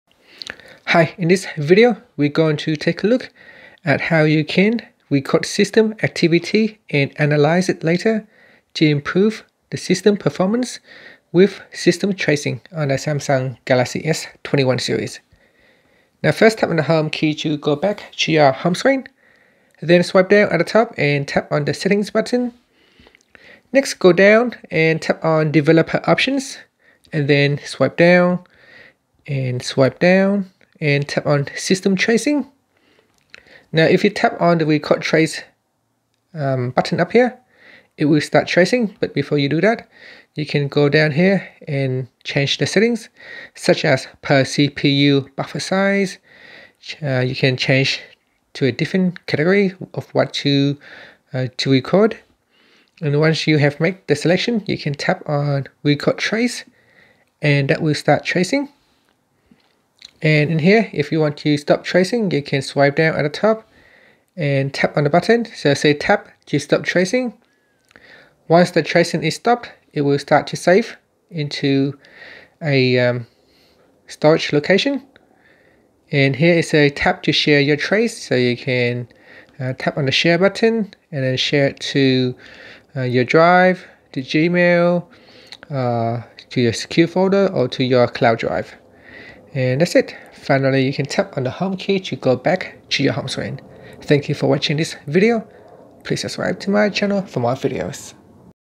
Galaxy S21/Ultra/Plus: How to Record System Activity
Learn how you can record the system activity and analyze it later to improve the system performance  with system tracing on the Galaxy S21/Ultra/Plus.